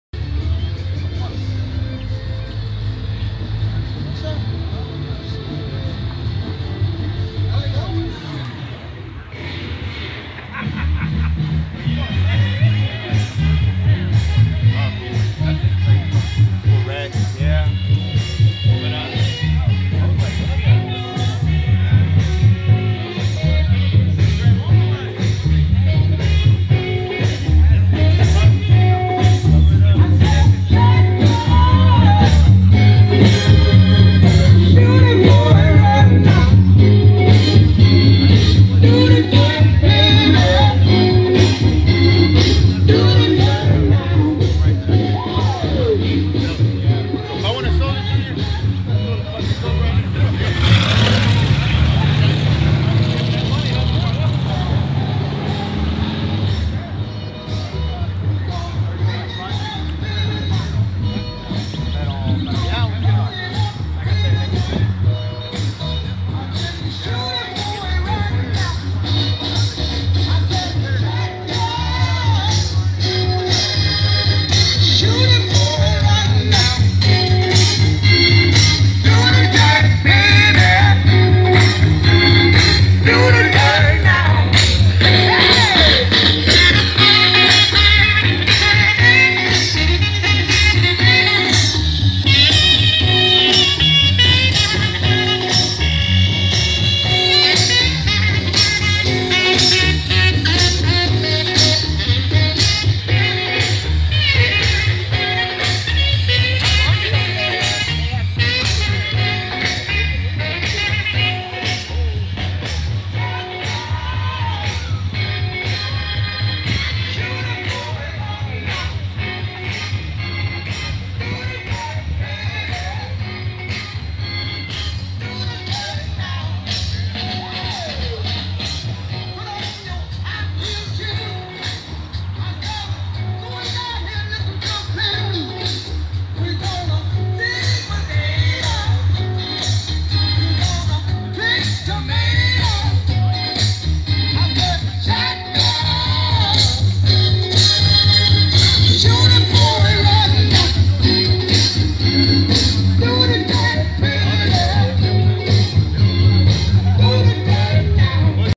IMPERIAL VALLEY LOWRIDERS
LOWRIDERS CHILLIN IN BRAWLEY CALIFORNIA